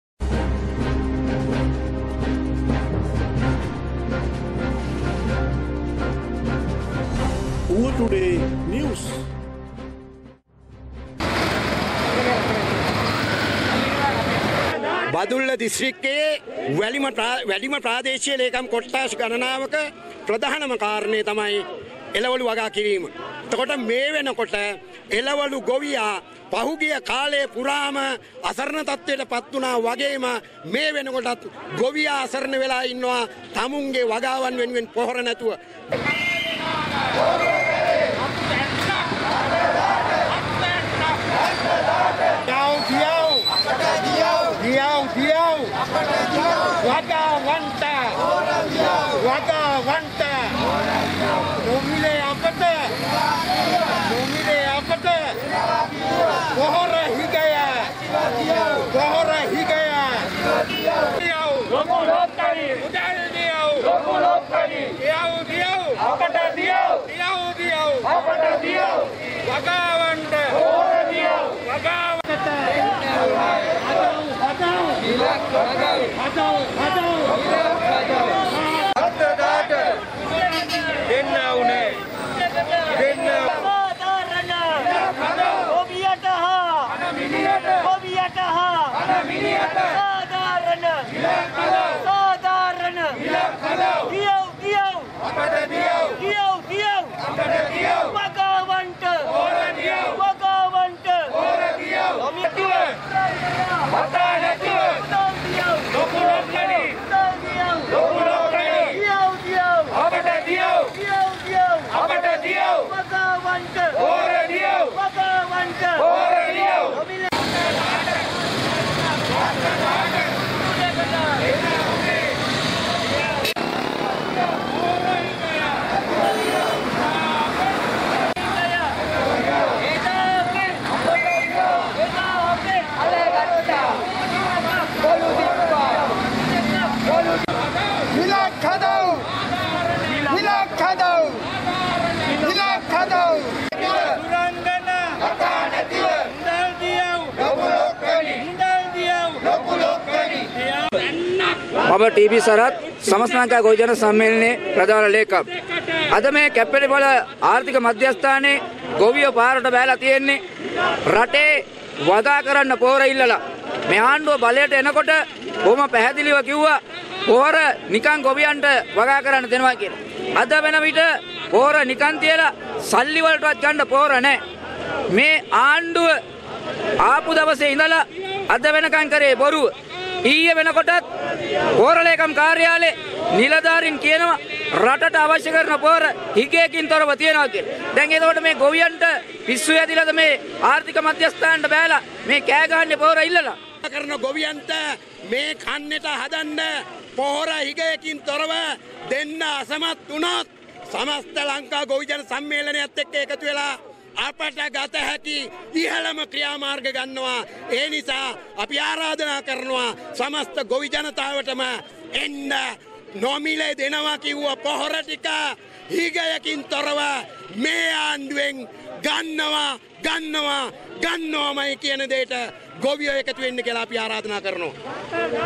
පොහොර දියව්-වැලිමඩ අල ගොවියෝ කැප්පෙටිපොල ආර්ථික මධ්‍යස්ථානය ඉදිරිපිට දී විරෝධතාවයක-fertilizer
මෙවර වගා කන්නයේ දී අර්තාපල් හා එළවළු වගාවන් හට නිසි ලෙස පොහොර නොමැති බව පවසා වැලිමඩ ගොවීන් කැප්පෙටිපොල ආර්ථික මධ්‍යස්ථානය ඉදිරිපිට දී  විරෝධතාවයක නිරත වූහ.වැලිමඩ ගොවි එකමුතුව මගින් මෙය  සංවිධානය කර තිබිනි.